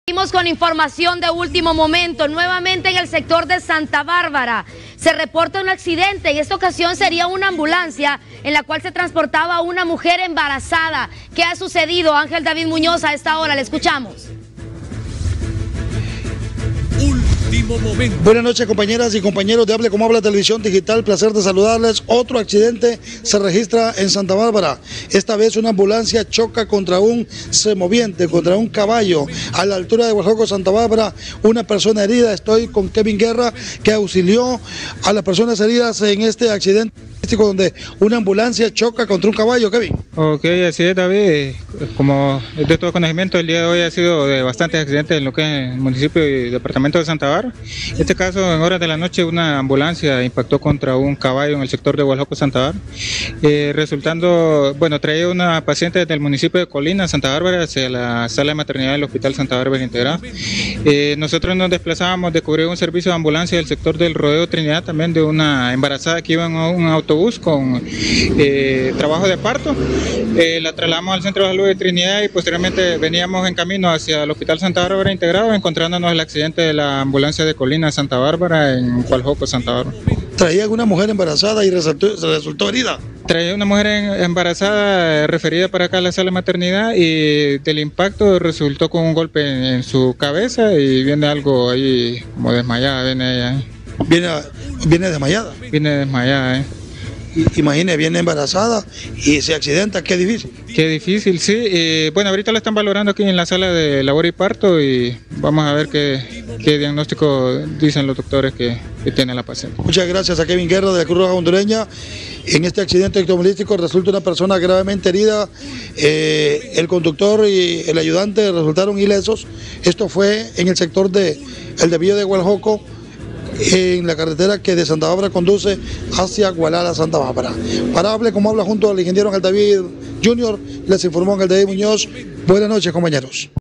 Ambulancia choca contra un animal en Gualjoco, Santa Bárbara
En Santa Bárbara se produjo un accidente en el que una ambulancia de la Cruz Roja Hondureña impactó con un caballo. El conductor y su acompañante resultaron ilesos.
No obstante, el vehículo de emergencia transportaba a una mujer embarazada que salió con golpes leves luego del choque.

Kevin Guerra, miembro de la Cruz Roja, dijo que, “traíamos a una paciente embarazada de Colinas Santa Bárbara y del impacto, resultó con un golpe en su cabeza, se desmalló. A ella se le está valorando para dar un diagnóstico de su estado de salud”.